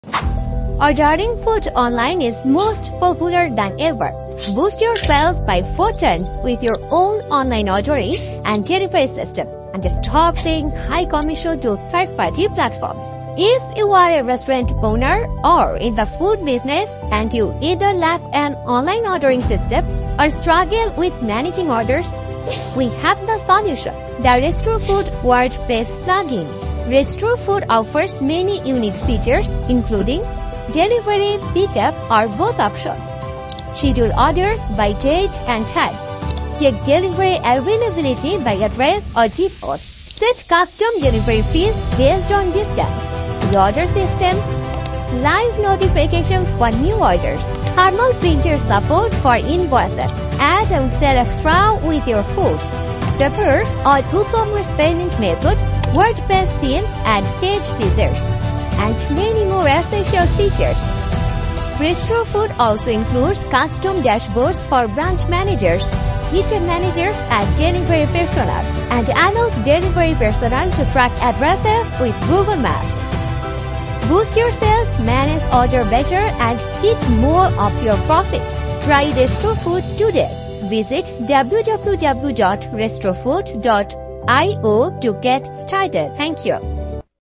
RestroFood - Boost your restaurant sales 4x with your own online food ordering and delivery system
Ordering food online is more popular than ever. Boost your sales by 4x with your own online ordering and delivery system, and stop paying high commissions to third-party platforms.

If you're a restaurant owner or in the food business, and you either lack an online ordering system or struggle with managing orders, we have the solution: the RestoFood WordPress plugin.

RestoFood offers many unique features, including:
- Delivery, pickup, or both options
- Schedule orders by date and time
- Check delivery availability by address or zip code
- Set custom delivery fees based on distance
- Pre-order system
- Live notifications for new orders
- Thermal printer support for invoices
- Add and sell extras with your foods
- Supports all WooCommerce payment methods, WordPress themes, and page builders
- And many more essential features

RestoFood also includes custom dashboards for Branch Managers, Kitchen Managers, and Delivery Personnel, and allows delivery personnel to track addresses with Google Maps.

Boost your sales, manage orders better, and keep more of your profits. Try RestoFood today!